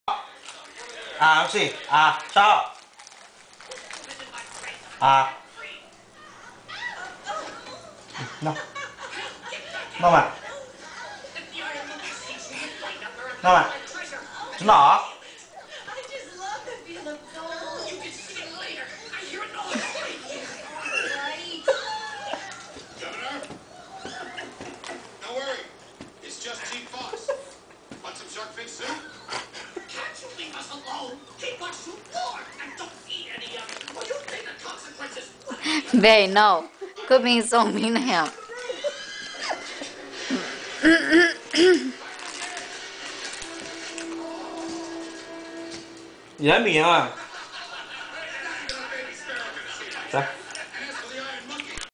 ethan's funny faces
Ethan's eating sour candy and making funny faces